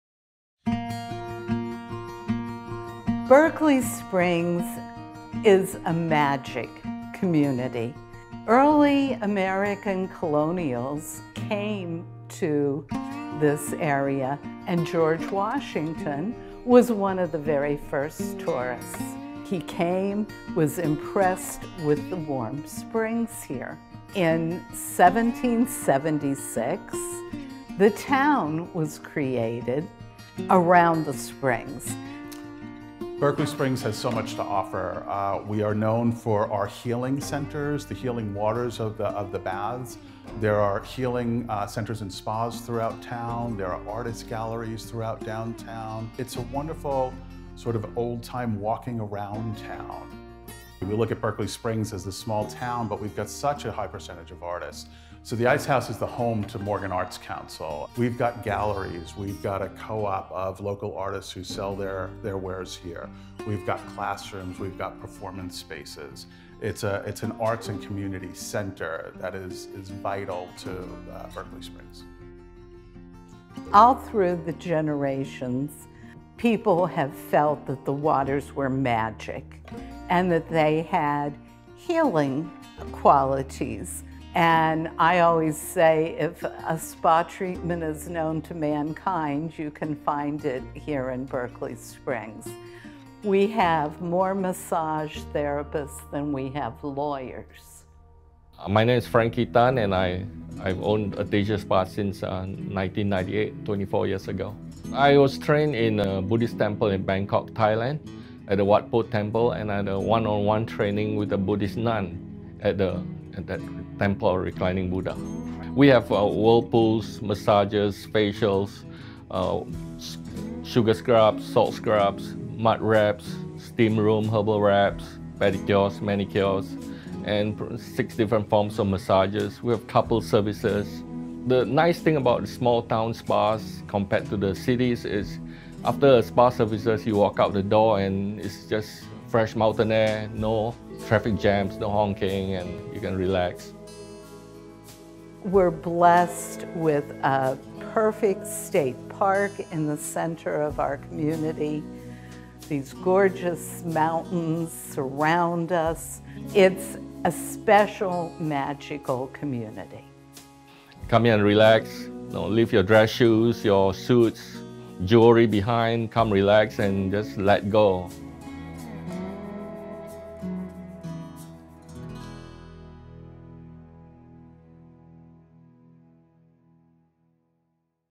Serene spa getaway outside of D.C.
Few places provide relaxation and serenity like a mountain town. No matter the time of year, escaping the city to spend time in Berkeley Springs’ crisp air is a pastime for Washingtonians and residents of other nearby cities.